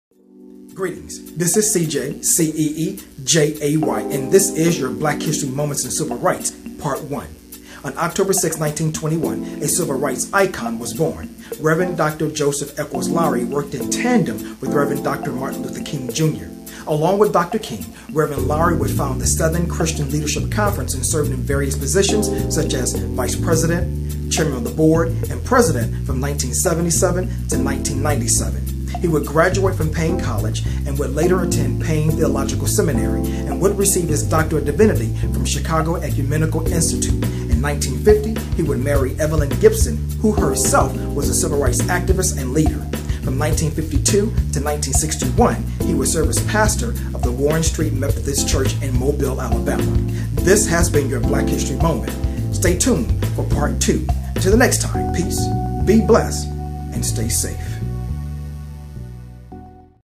Black History Moment Part One: Rev. Joseph Lowery
Black History Moment in Civil Rights, Part One honoring the legacy of Rev. Joseph Lowery.